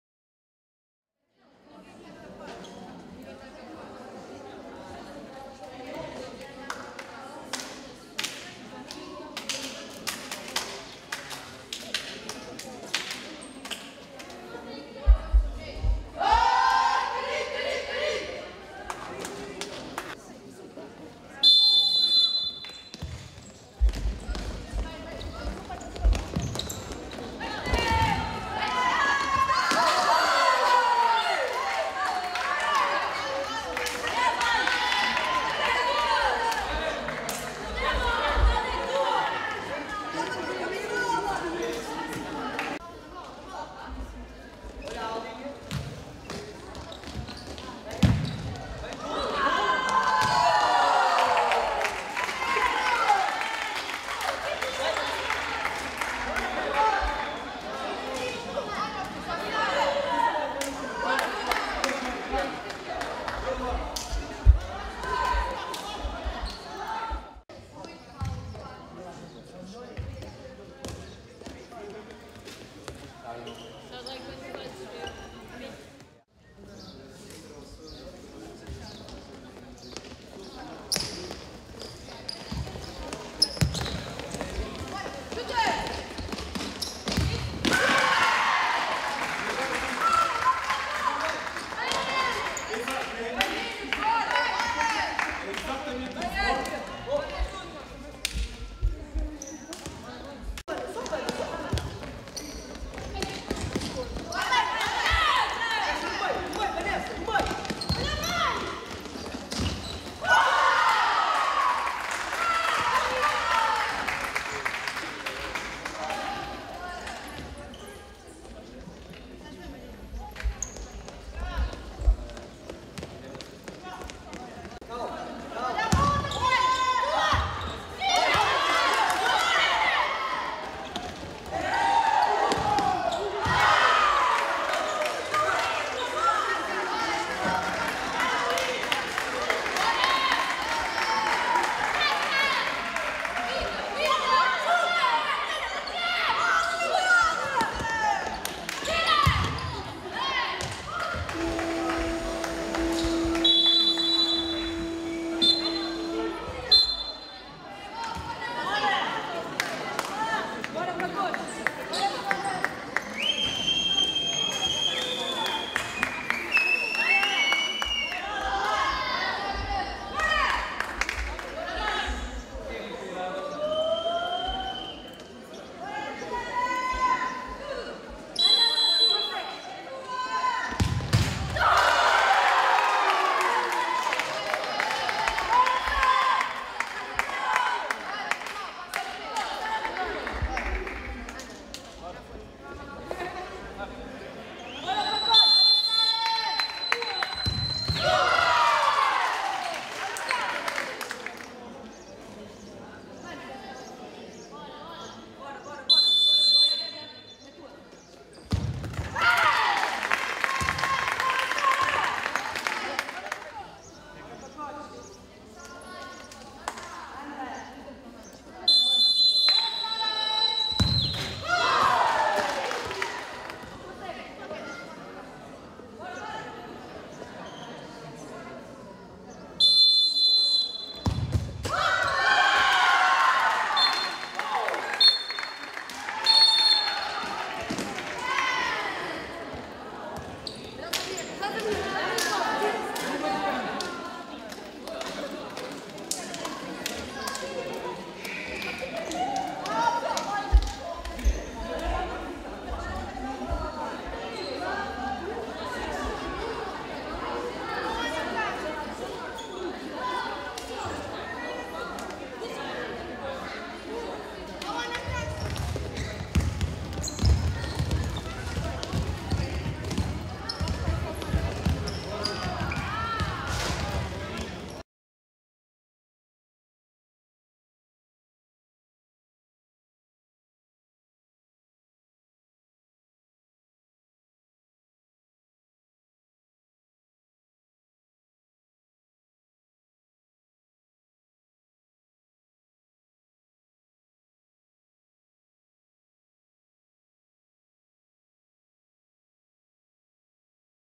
IV Torneio Catarina Câncels - Jogo 3º/4º Lugar: CARNIDE 2x2(GP) PREGANÇA
IV Torneio Catarina Câncels
Jogo 3º/4º Lugar

CARNIDE CLUBE x PREGANÇA

Domingo, 16 de Junho de 2019 às 12h50